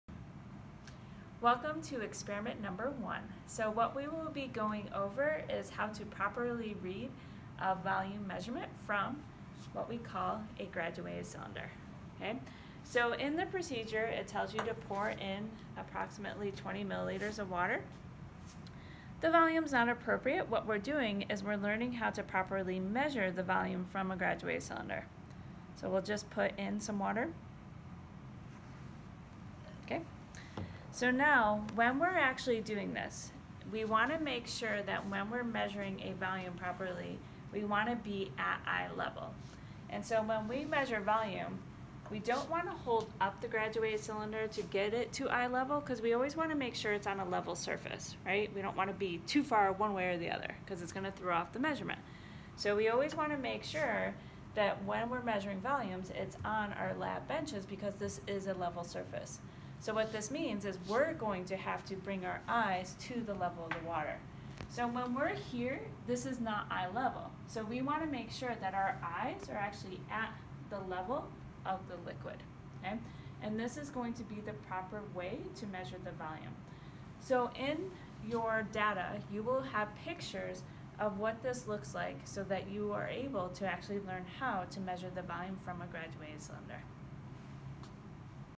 chem152Lexp1ptcparallax
video of experiment 1 part C, reading a graduated cylinder for chem 152L at Mesa College